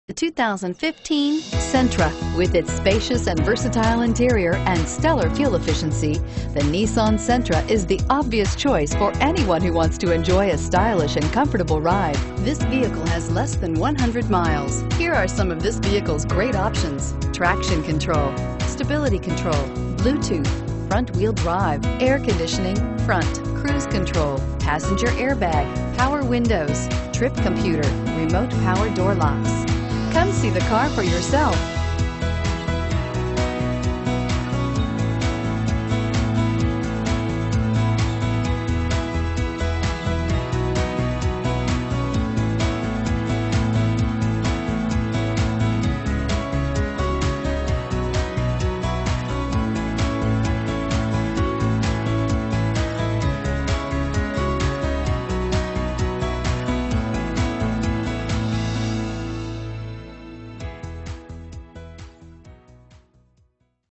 2015 Nissan Sentra Chico CA 7006
2015 Nissan Sentra S

Need gas? I don't think so. At least not very much! 39 MPG Hwy!!! NEW LOW PRICE... Safety equipment includes: ABS, Traction control, Curtain airbags, Passenger Airbag, Stability control...Other features include: Bluetooth, Power locks, Power windows, CVT Transmission, Air conditioning...